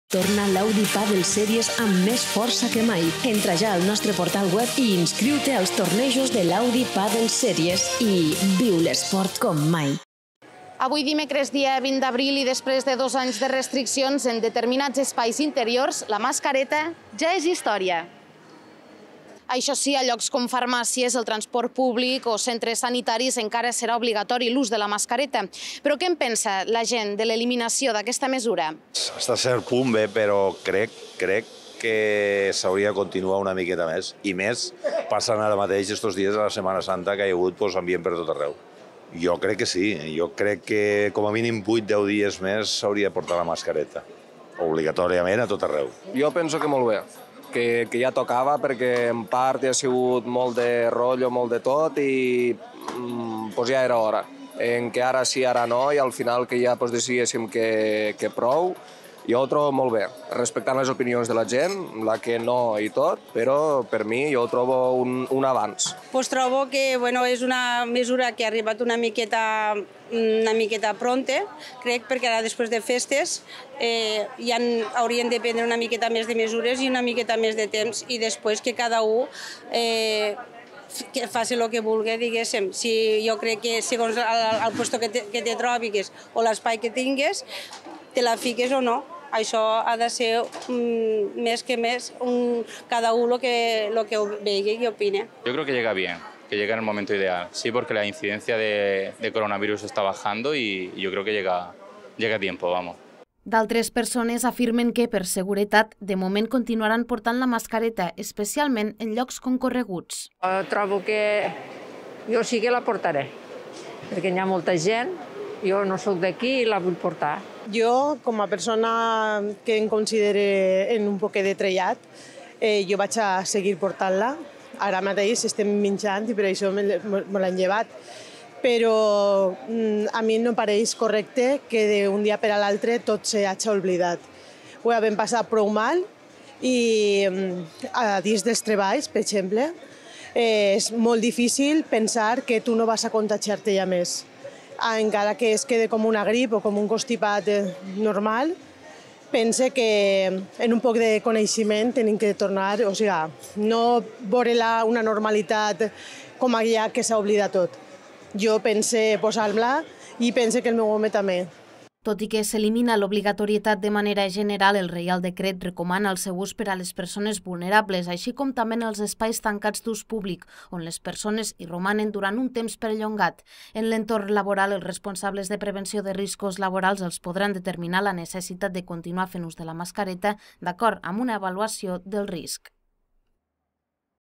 Adéu a la mascareta en interiors després de dos anys de pandèmia. Què opinen al mercat de Tortosa?
El Butlletí Oficial de l’Estat (BOE) ha publicat aquest dimecres 20 d’abril el reial decret que estableix la fi de l’obligatorietat de dur mascareta en interiors, amb algunes excepcions. Entre aquestes, el transport públic, farmàcies, centre sanitaris i visitants i treballadors de residències i centres sociosanitaris. La mesura ha estat ben rebuda tot i que genera dubtes que es posi fi a l’ús obligatori de la mascareta just després de la Setmana Santa. Ens ho explicat la nostra companya Alba Mor des del mercat de Tortosa

D’altres persones afirmen que, per seguretat, de moment continuaran portant la mascareta, especialment en llocs concorreguts.

Tot i que s’elimina l’obligatorietat de manera general, el reial decret recomana el seu ús per a les persones vulnerables, així com també en els espasi tancats d’ús públic on les persones hi romanen durant un temps perllongat. En l’entorn laboral, els responsables de prevenció de riscos laborals els podran determinar la necessitat de continuar fent ús de la mascareta, d’acord amb una avaluació del risc.